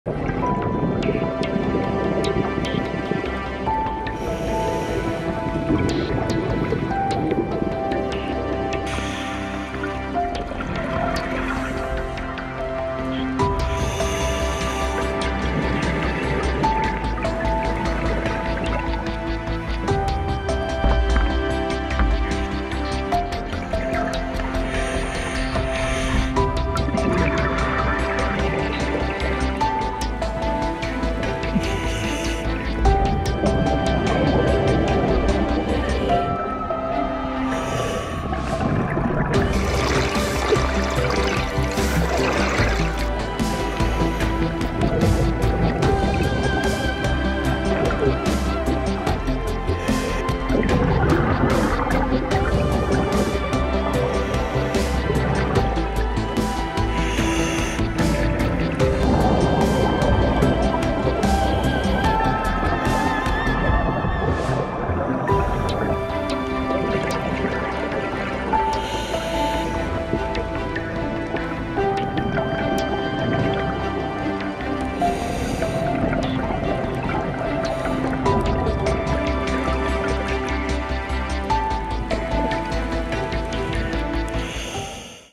PHILIPPINES WORLD WAR II SHIPWRECK DIVING UNDERWATER
During World War II in 1944, an American air raid attacked 24 Japanese ships near Coron in the Philippines. Now you can dive 12 of the shipwrecks. Check out the old artillery and new reefs in this underground museum.